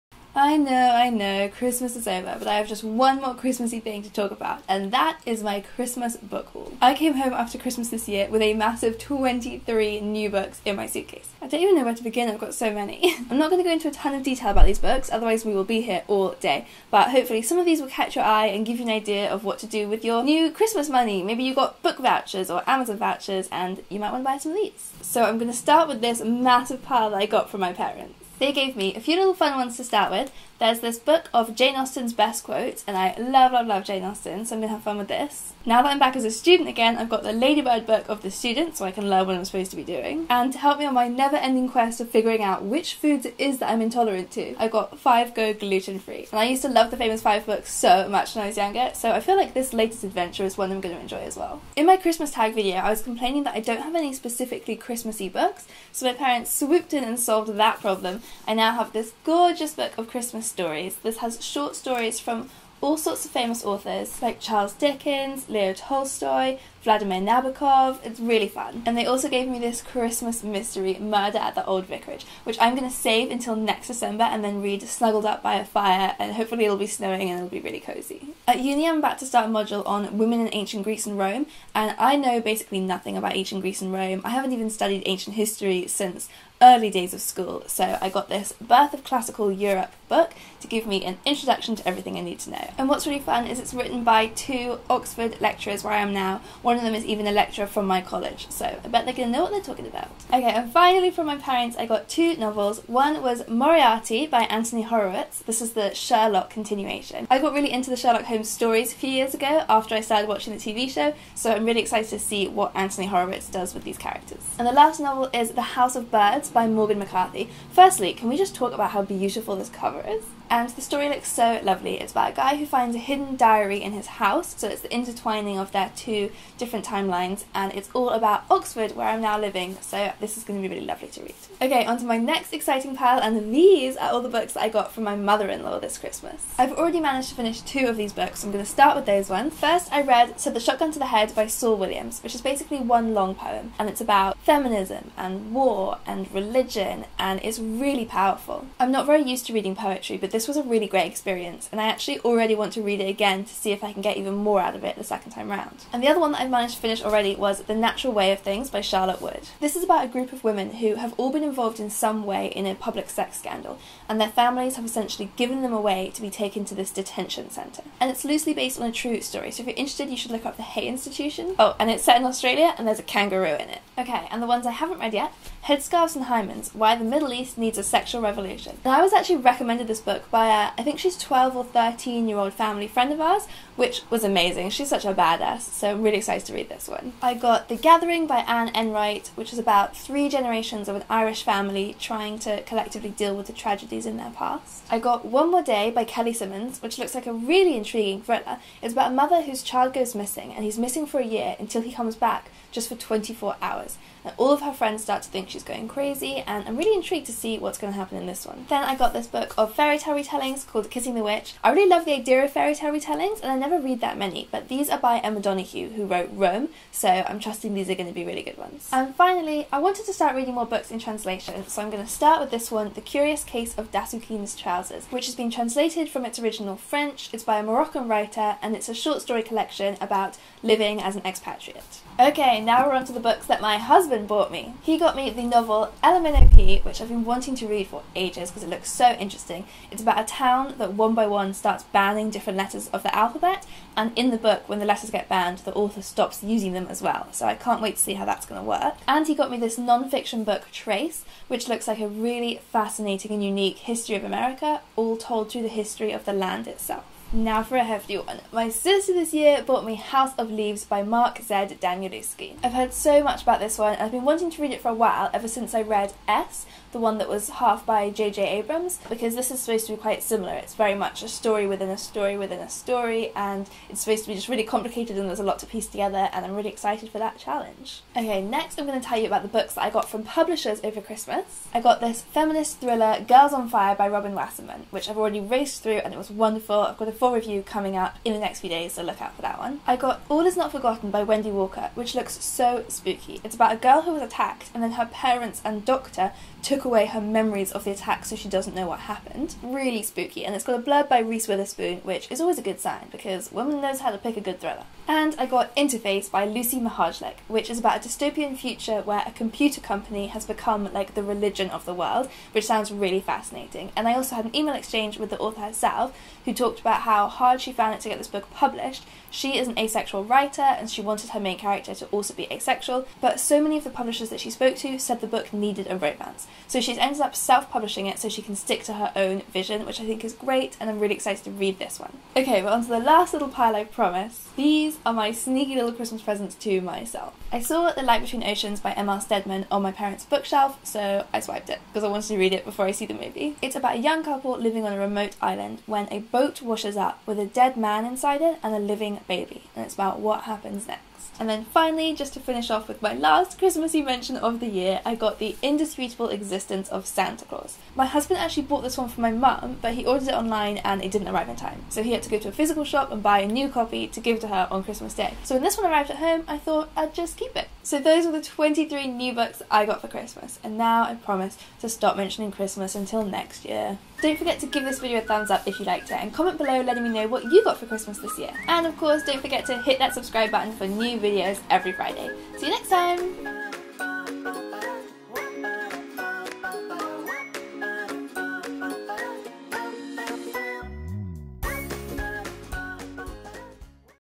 🎅 FATHER CHRISTMAS BROUGHT ME A BOOK HAUL 🎅 | Drinking By My Shelf
I got a massive book haul of 23 books for Christmas this year and heeeeere they are

~*IF YOU WANNA BUY SOME BOOKS HERE'S A REAL COOL WEBSITE*~

~*BOOKS MENTIONED*~
Jane Austen in her own words - Annie Bullen
How It Works: The Student - Jason Hazely & Joel Morris
Five Go Gluten Free - Bruno Vincent
Christmas Stories - ed. Diana Secker Tesdell
Murder at the Old Vicarage - Jill McGown
Moriarty - Anthony Horowitz
The House of Birds - Morgan McCarthy
.Said the Shotgun to the Head - Saul Williams
Headscarves and Hymens - Mona Eltahawy
The Gathering - Anne Enright
One More Day - Kelly Simmons
Kissing the Witch - Emma Donoghue
The Curious Case of Dassoukine's Trousers - Fouad Laroui
The Natural Way of Things - Charlotte Wood
Ella Minnow Pea - Mark Dunn
Trace - Lauret Savoy
House of Leaves - Mark Z. Danielewski
Girls on Fire - Robin Wasserman
All Is Not Forgotten - Wendy Walker
Interface - Lucy Mihajlich
The Light Between the Oceans - M.L. Steadman
The Indisputable Existence of Santa Clause - Hannah Fry & Thomas Oleron Evans

I got Twitter, Instagram, Goodreads, all that good shit. Username is always emmaoulton